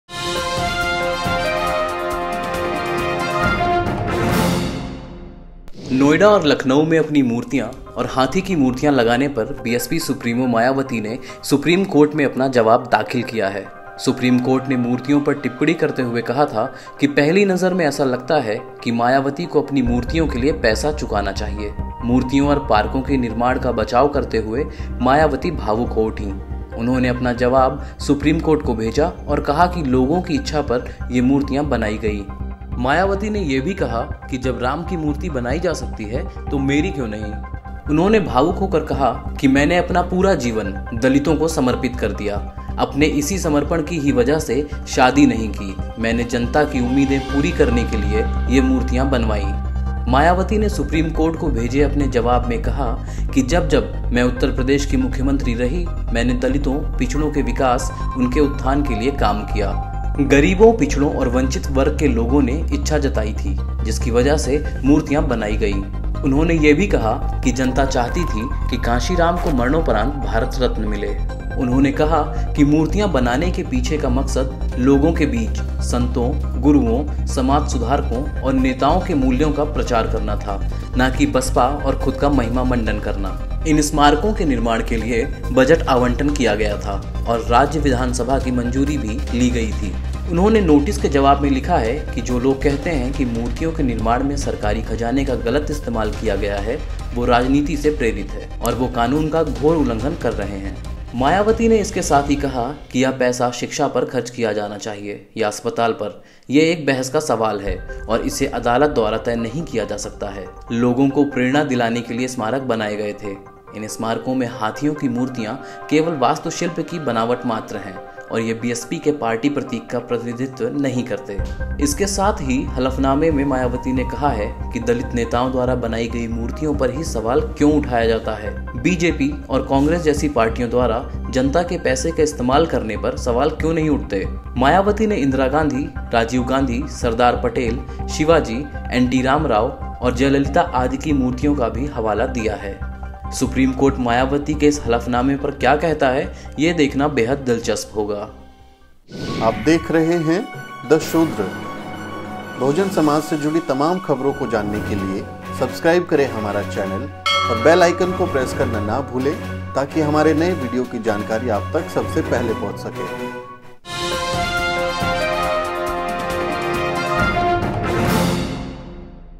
Mayawati Statue : जब राम की मूर्ति बन सकती है तो मेरी क्यों नहीं? मायावती का पूरा जवाब सुनिए
नोएडा और लखनऊ में अपनी मूर्तियां और हाथी की मूर्तियां लगाने पर बीएसपी सुप्रीमो मायावती ने सुप्रीम कोर्ट में अपना जवाब दाखिल किया है। मायावती ने ये भी कहा कि जब राम की मूर्ति बनाई जा सकती है तो मेरी क्यों नहीं ?
Mayawati: If Ram statue OK, why not mine?